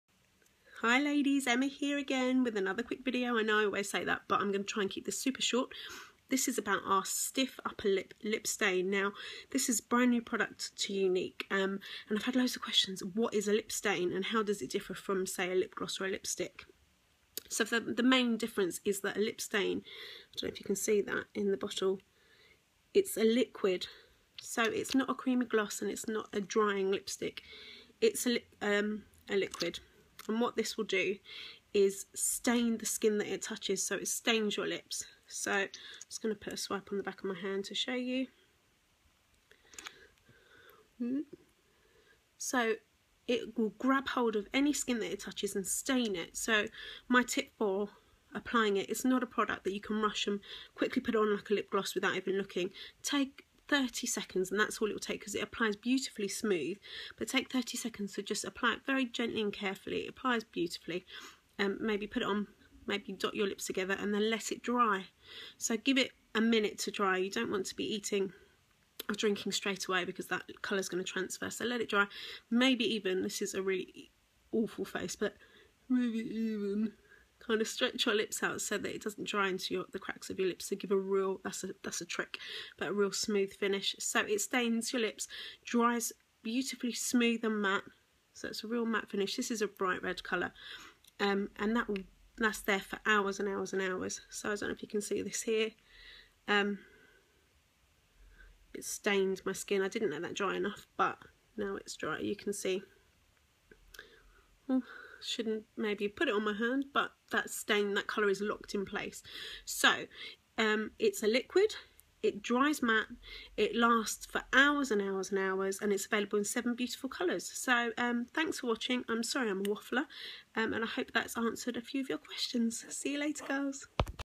What is a Lip Stain? Younique's Stiff Upper Lip Lip Stain - how to apply and how does it look?
I've had a lot of people ask me "what exactly is a lip stain" so here's a quick video! Brand new to Younique are the Stiff Upper Lip Lip Stains - available in 7 stunning shades. Smooth matte & super long wearing! Budge proof, smudge proof vibrant colour!